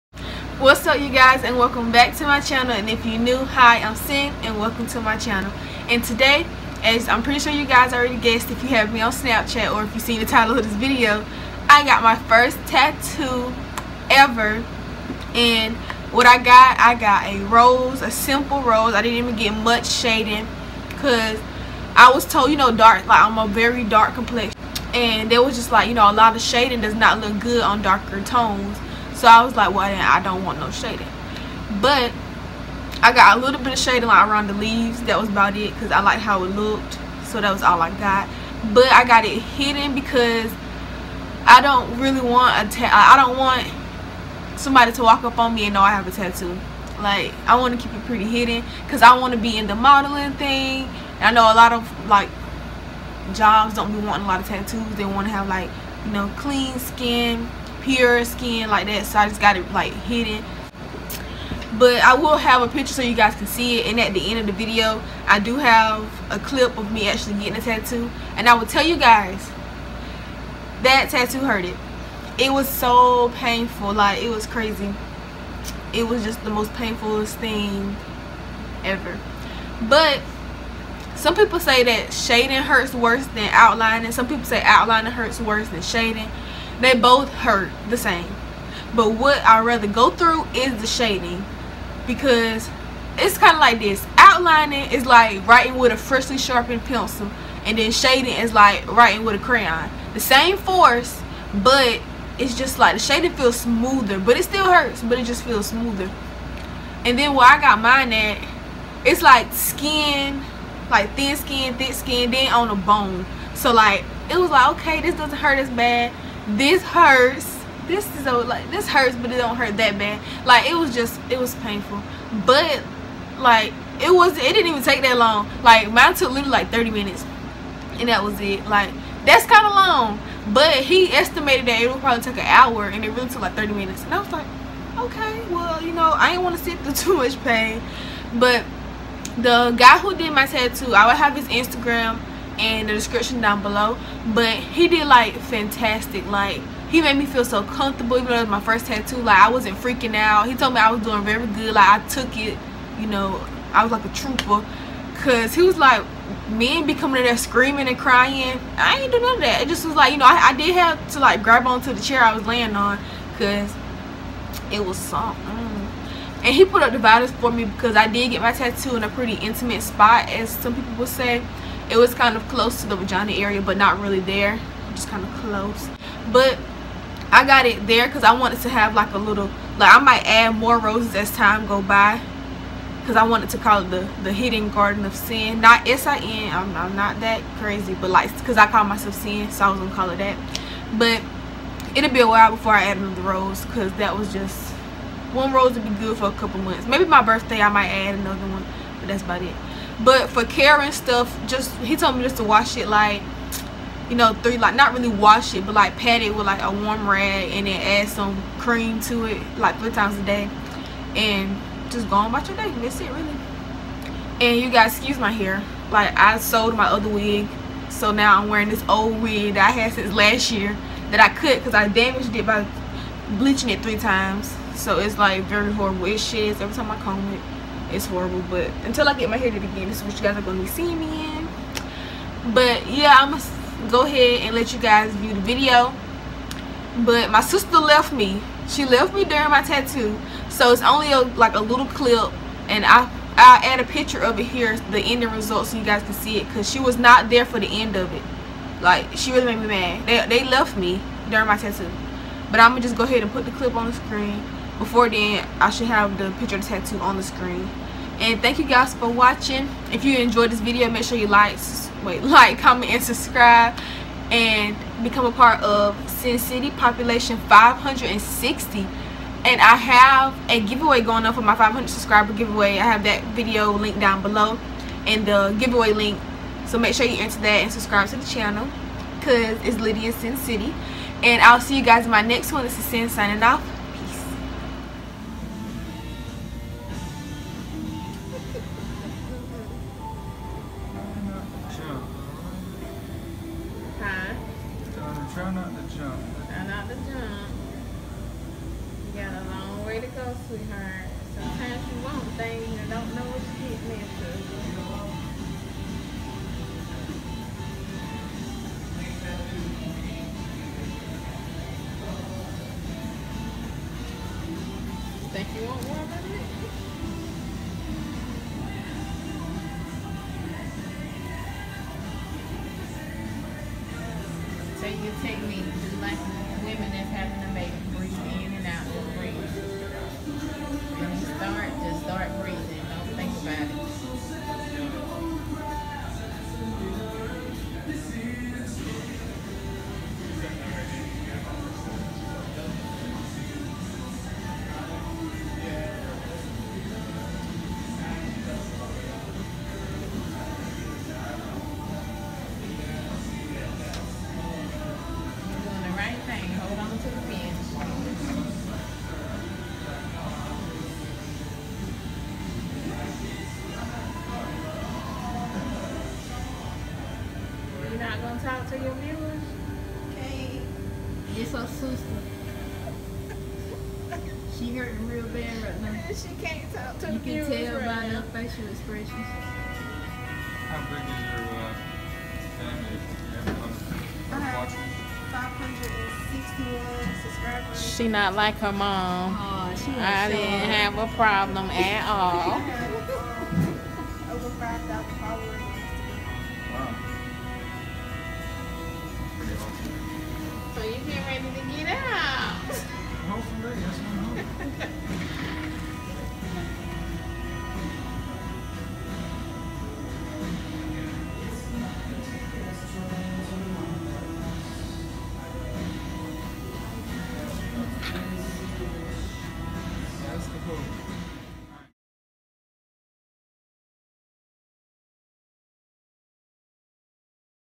Getting My First Tattoo!!! | Live Footage!!!
⬇️Check for More Info⬇️

 My First Tattoo Experience With Live Footage!!!

Tattoo Artist Instagram:
Nick_santoro2611

Porkchop Tattoo Shop
Florence, SC

Please excuse the quality it will improve soon!!

Support Me!

DoDo Lashes: simplygawwwjuss

Give $2 Get $2 with my Mercari Code:
CTMFPF